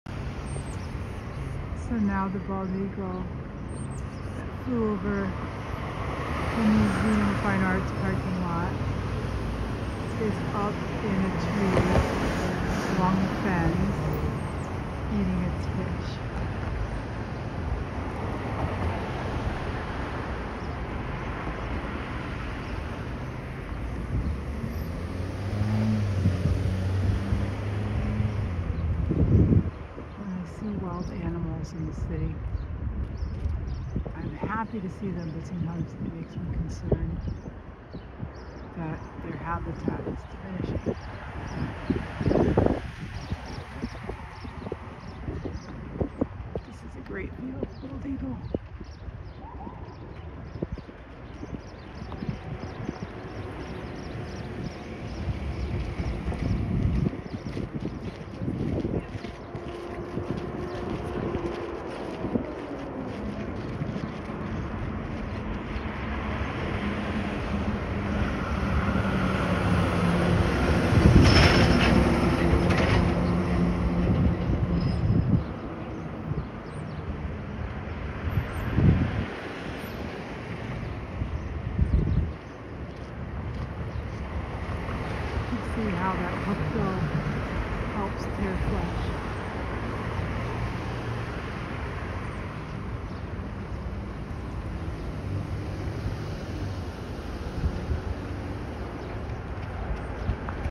Guess what flew over me today?
As I ate lunch outside the Boston MFA, a large bird flew over me with a fish; it was a Bald Eagle! I followed it and got to watch it eat lunch.